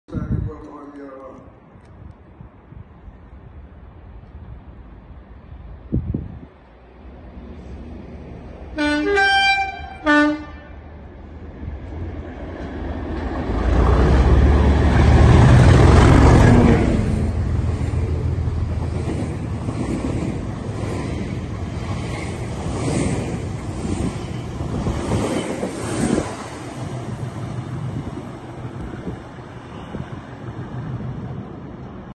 313 Scrap Move (Class 37) @ Three Bridges (10/03/23)
The first three units to be withdrawn from public service, here is the first drag move passing through Three Bridges, with a couple of tones!